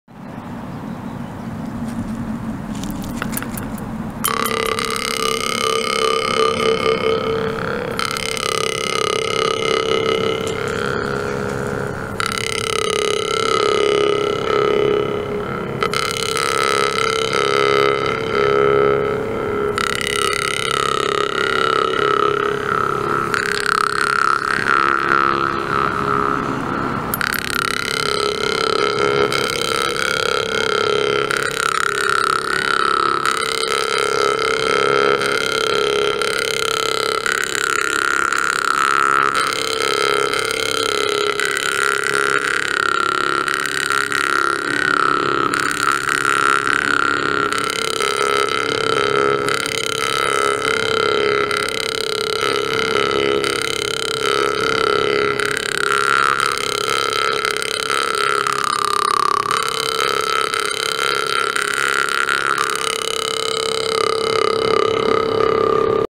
One of the most selling model, jaw harp "Ultrabass Plus" with very low and unique base tone
One of the most selling model, jaw harp "Ultrabass Plus" with very low and unique base tone (30-35 Hz). Sound is clear, loud and ultralow. Size 150 mm. Echo sound effect added at video
This is top quality instrument made with a great accuracy in an austere design. This jew's harp is my work, so you can get the instrument directly from the master. This jew's harp is recommended for both beginners and advanced players.

Check out the more info and sound
VladHarps.com